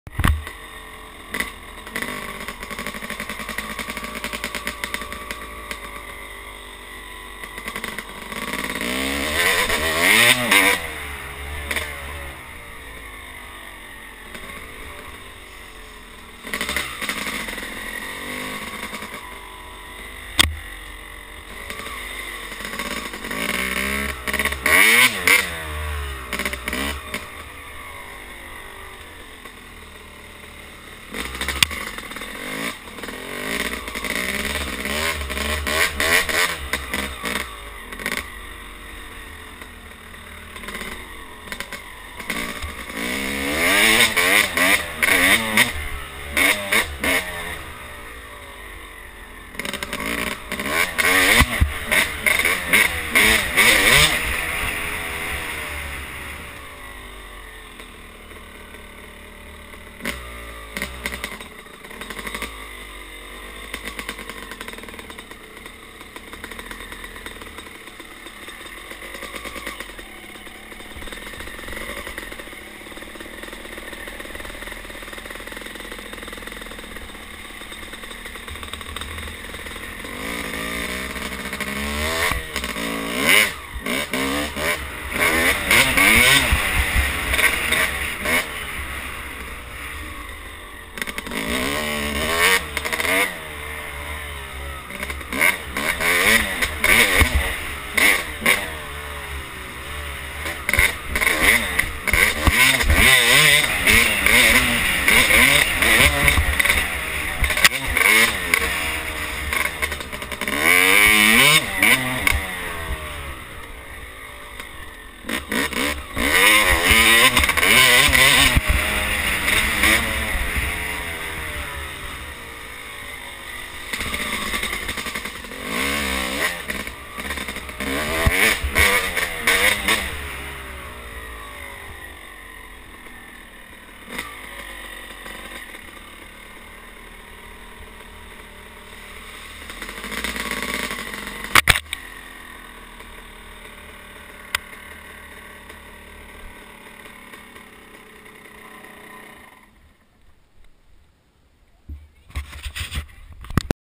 YZ at the track.MP4
Well I was going to turn some laps, but turns out it was pretty flooded!